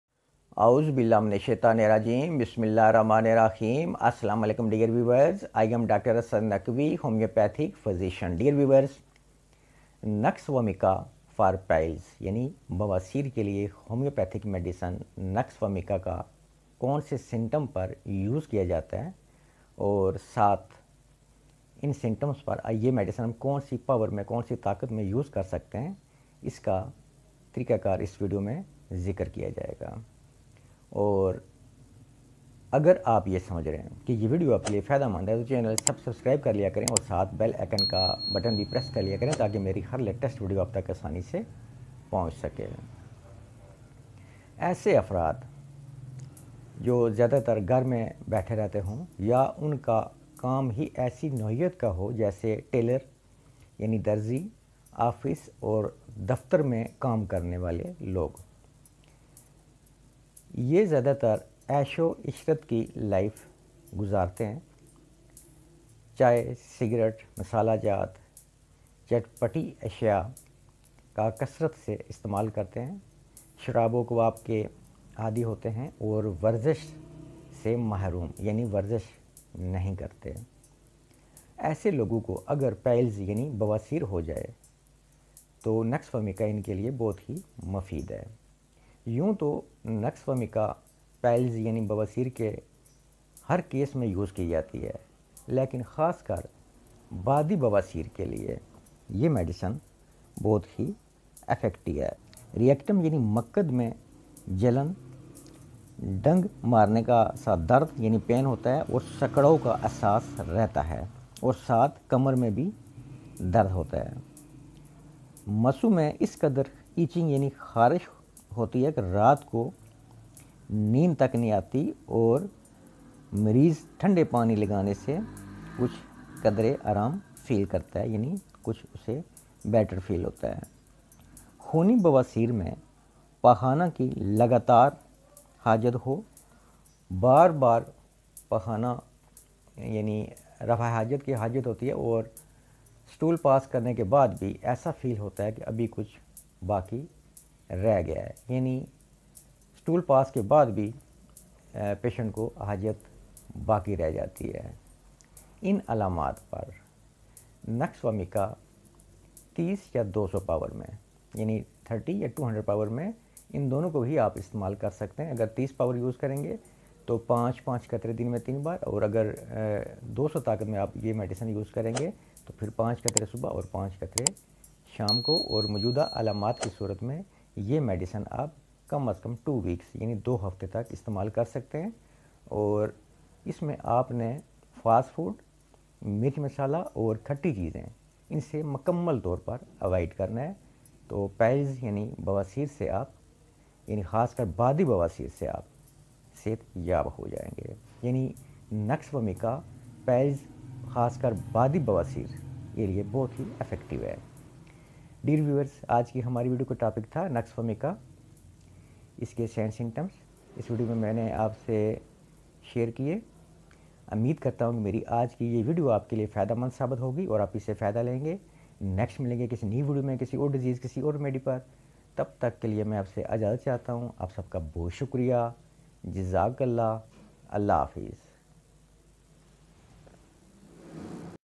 Nux vomica 30,200 ll for piles with symptoms & use ll Dr Asad Abbas
Asalam o Alikum i am Dr Asad Abbas Naqvi.This channel is all about health,topics related to health.Homoeopathy Treatment.

Asalam o Alikum..this video mein nux vomica 30,200 for piles with symptoms benefits & use share kya gya hai watch this video thnx